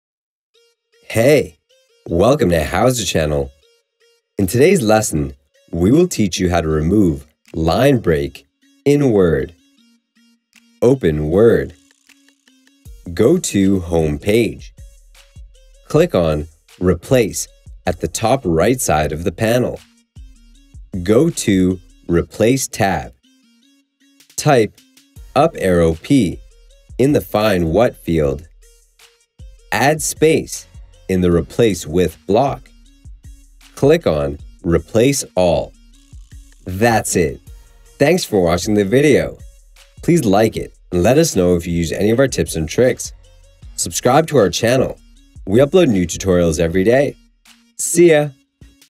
How to Remove Line Breaks in Word
In today's lesson, we will teach you how to remove line break in Word.
Open Word.
Go to Home page. Click on Replace at the top right side of the panel. Go to Replace tab. Type ^p in the Find what field. Add space in the Replace with block. Click on Replace All.